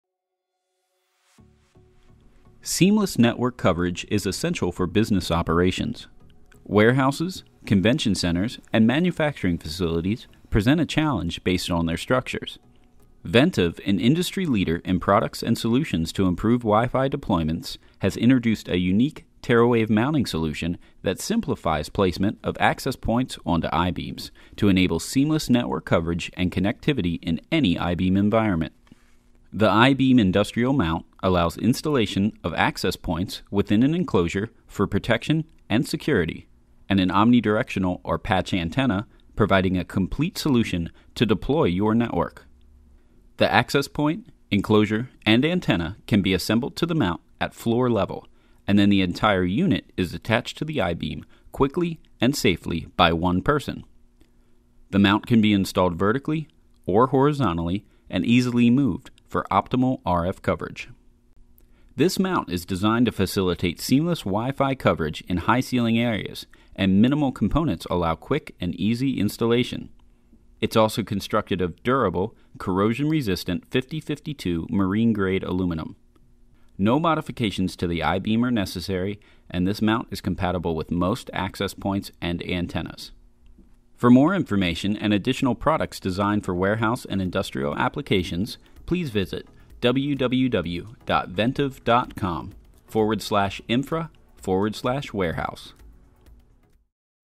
Ventev's I-Beam Industrial Mount
The low profile design with minimal components makes installation quick and easy. In addition, the ease of installation allows the mount to be moved quickly to optimize RF coverage.